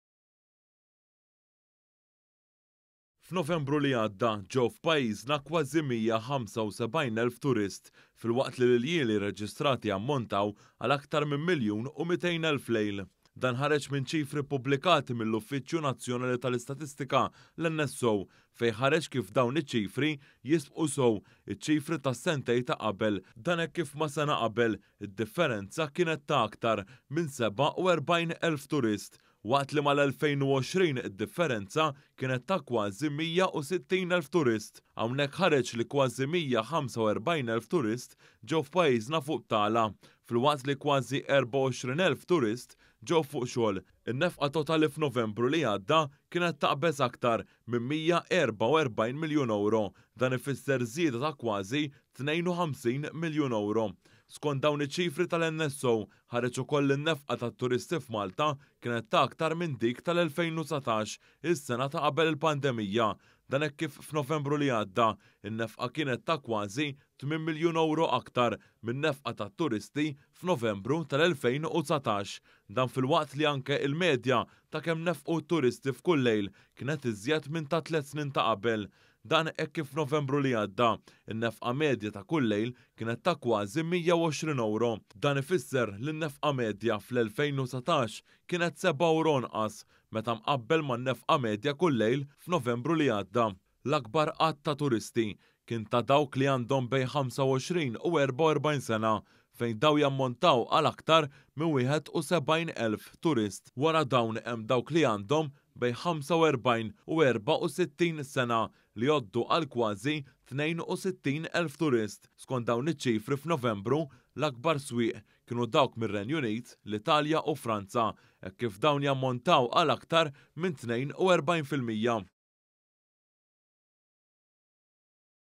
F’NOVEMBRU 2022 IN-NEFQA TAT-TURISTI KIENET AKTAR MINN NOVEMBRU 2019
ONE.COM.MT • Skont ċifri tal-NSO, f’Novembru li għadda ġew f’pajjiżna kważi mija ħamsa u sebgħin elf turist.

Minn dawn iċ-ċifri ħareġ ukoll li f’Novembru li għadda, in-nefqa kienet iżjed min-nefqa tat-turisti fl-elfejn u dsatax. Hawnhekk ħareġ li n-nefqa f’Novembru li għadda, kienet taqbeż il-mija erbgħa u erbgħin ewro.

FILMAT: ARKIVJI

F’Novembru li għadda ġew f’pajjizna kważi mija ħamsa u sebgħin elf (174,460) turist, filwaqt li l-iljieli rreġistrati jammontaw għal aktar miljun u mitejn elf (1,203,802) lejl.

Dan ħareġ minn ċifri ppublikati mill-Uffiċċju Nazzjonali tal-Istatistika, l-NSO, fejn ħareġ kif dawn iċ-ċifri jiżbqu sew iċ-ċifri tas-sentejn ta’ qabel.

Dan hekk kif ma’ sena qabel id-differenza kienet ta’ aktar minn sebgħa u erbgħin elf (47,277) turist, waqt li mal-2020, id-differenza kienet ta’ kważi mija u sittin elf (159,524) turist.

Hawnhekk ħareġ li kważi mija ħamsa u erbgħin elf (144,631) turist ġew f’pajjiżna fuq btala, filwaqt li kważi erbgħa u għoxrin elf (23,615) turist ġew fuq xogħol. GRAFIKA 3

In-nefqa totali f’Novembru li għadda kienet taqbeż aktar minn mija erbgħa u erbgħin miljun ewro (€144 miljun). Dan ifisser żieda ta’ kważi tnejn u ħamsin miljun ewro (€51.6 miljun).

Skont dawn iċ-ċifri tal-NSO ħareġ ukoll li n-nefqa tat-turisti f’Malta kienet ta’ aktar minn dik tal-2019; is-sena ta’ qabel il-pandemija. Dan hekk kif f’Novembru li għadda, in-nefqa kienet ta’ kważi €8 miljun aktar min-nefqa tat-turisti f’Novembru tal-2019.

Dan filwaqt li anki l-medja ta’ kemm nefqu t-turisti f’kull lejl kienet iżjed mit-tliet snin ta’ qabel. Dan hekk kif f’Novembru li għadda, in-nefqa medja ta’ kull lejl kienet ta’ kważi €120. Dan ifisser li n-nefqa medja fl-2019 kien €7 anqas meta mqabbel man-nefqa medja kull lejl f’Novembru li għadda.

L-akbar għadd ta’ turisti kien ta’ dawk li għandhom bejn 25 u 44 sena, fejn dawn jammontaw għal aktar minn wieħed u sebgħin elf (71,021) turist. Wara dawn hemm dawk li għandhom bejn 45 u 64 sena, li jgħoddu għal kważi tnejn u sittin elf (61,791) turist.

Skont dawn iċ-ċifri, f’Novembru, l-akbar swieq kienu dawk mir-Renju Unit, l-Italja u Franza, hekk kif dawn jammontaw għal aktar minn 42 fil-mija.